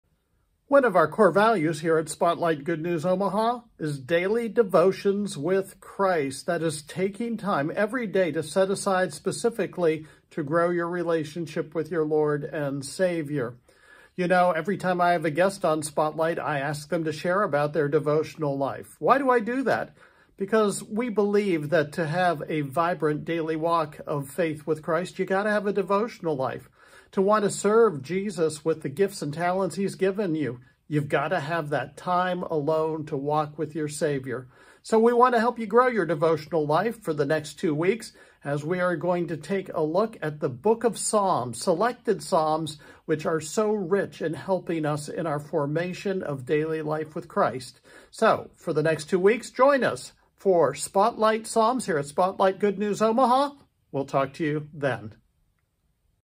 Next Week on Spotlight—Spotlight Psalms as Devotional Reading
The Old Testament book of Psalms is a favorite of many Christians because it speaks from the heart to our Lord. The Psalms are devotions that express the full range of our experience with God. For the next two weeks , we’ll explore selected Psalms to help us grow in our daily lives walk of faith. Join us!